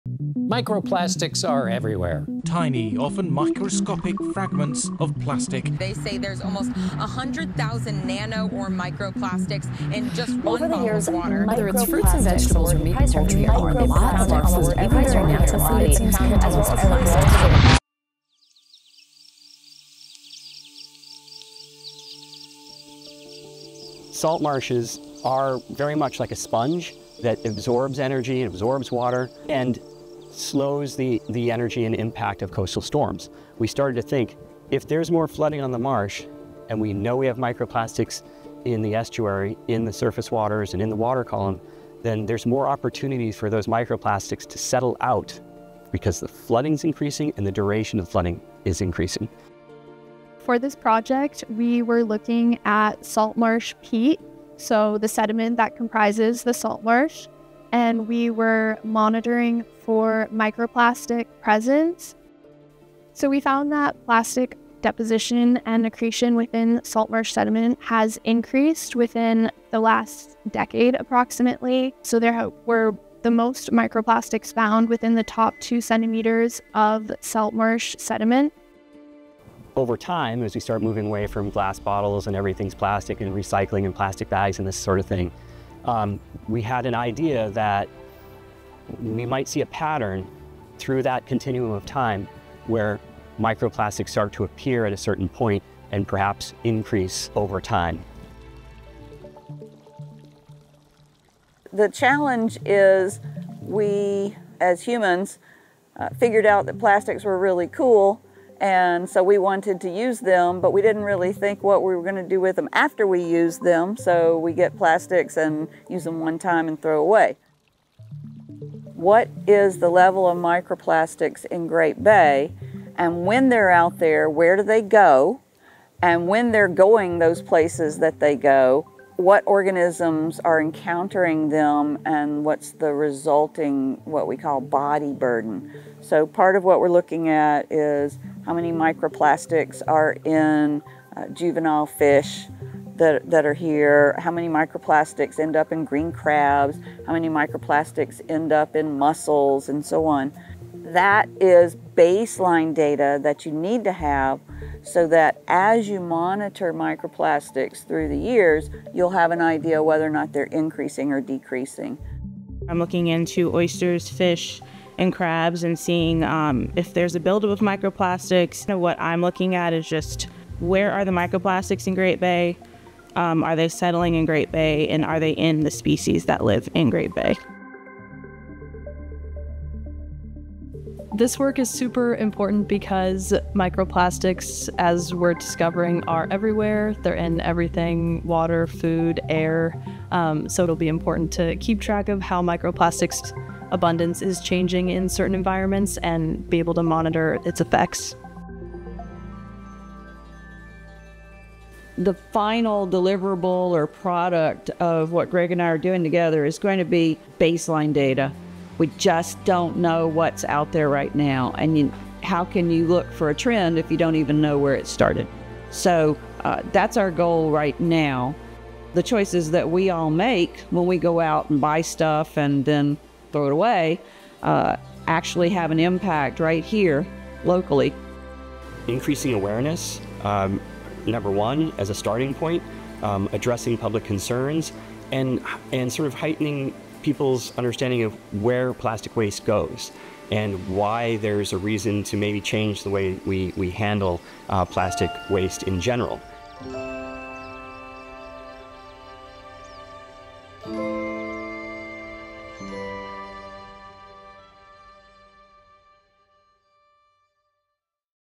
Could microplastics be a problem for New Hampshire's estuaries
At the University of New Hampshire, Bonnie Brown, professor of ecological genetics, and Gregg Moore, associate professor of coastal restoration and resilience, along with two graduate students and several undergrads, are working to establish baseline data on microplastics in New Hampshire’s estuaries. These coastal regions, which provide critical nesting and breeding habitats for many animal species and serve as important ecosystems for cleaning and filtering ocean waters, are also known to trap and retain microplastics.